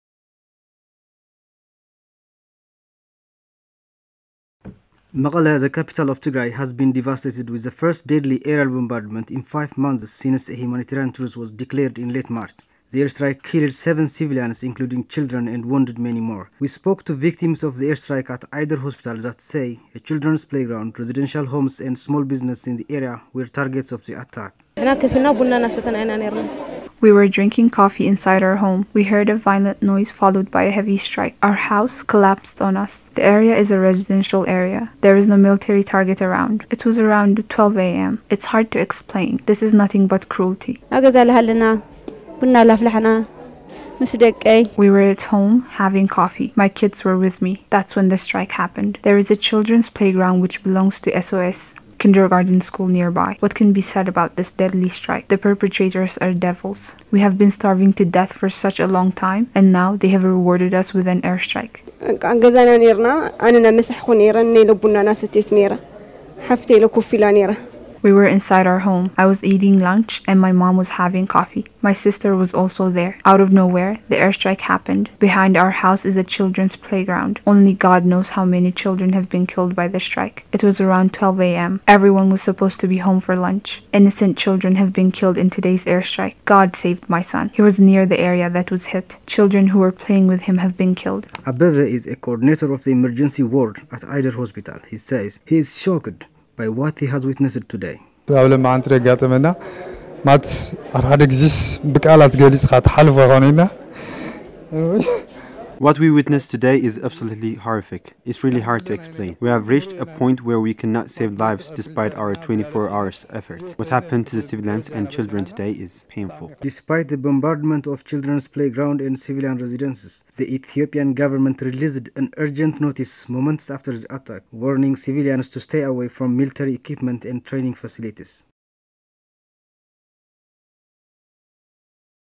Attacco aereo di un asilo a Mekelle , Tigray Etiopia il 26 agosto 2022
Servizio di Tigrai TV